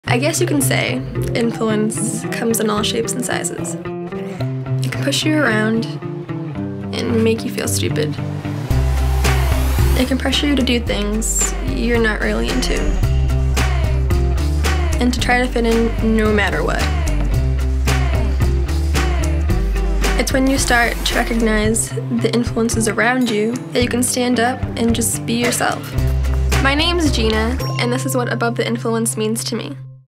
Above the Influence | Made by Geena
We asked teens across the country to share what Above the Influence means to them, and to pitch us their own ideas for a new commercial. Then we gave everyone the chance to vote and pick a winner.

Introducing the result of everyone's hard work... Made by Geena!

Above the Influence can mean different things to different people; the best place to explore what it could mean to you is